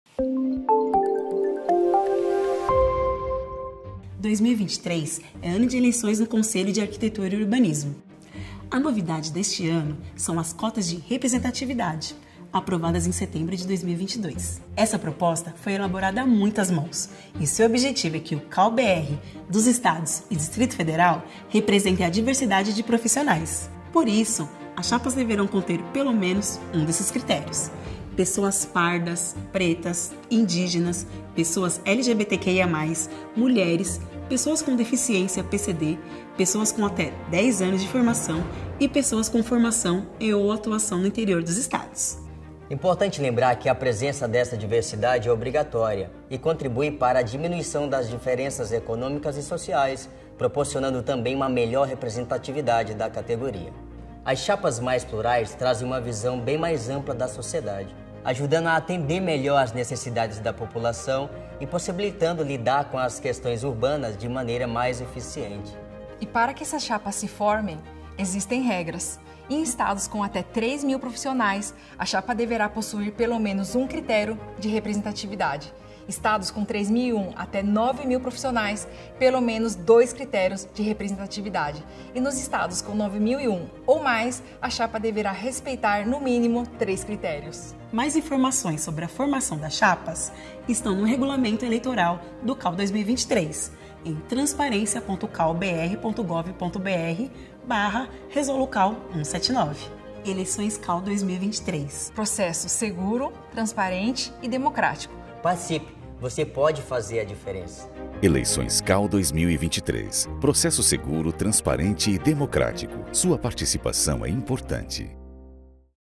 Eleições 2023: Como Cumprir as Cotas de Diversidade
Eleições do CAU 2023: Você sabe como cumprir as cotas de diversidade nas chapas?

Novidade deste ano, as cotas de diversidade visam promover a inclusão e garantir uma representatividade mais justa e equitativa em nossa profissão. Com mais espaço para grupos historicamente sub-representados, vamos fortalecer a diversidade de perspectivas em nosso campo de atuação, com mais mulheres, pessoas negras, indígenas, LGBTQIA+, pessoas com deficiência e profissionais com atuação no interior.

O cálculo das cotas de diversidade é feito de acordo com a quantidade de profissionais ativos registrados:

· Até 3.000 profissionais: Pelo menos 1 critério de representatividade

· 3.001 a 9.000 profissionais: Pelo menos 2 critérios de representatividade, não necessariamente na mesma vaga

· 9.001 ou mais profissionais: Pelo menos 3 critérios de representatividade, não necessariamente na mesma vaga.

· Além disso, 30% das vagas no primeiro terço devem atender a pelo menos 1 critério de representatividade.

Nosso compromisso é garantir que as chapas atendam a um mínimo de critérios de representatividade, assegurando que pelo menos um número de candidatos cotistas integre cada chapa.

Essa distribuição busca garantir que as diferentes regiões do Brasil sejam representadas de forma equitativa, dando voz a uma ampla variedade de perspectivas e experiências na construção do futuro do CAU.

As Eleições do CAU vão definir o futuro da Arquitetura e Urbanismo.

Faça parte da transformação, vamos juntos construir um CAU mais diverso e democrático!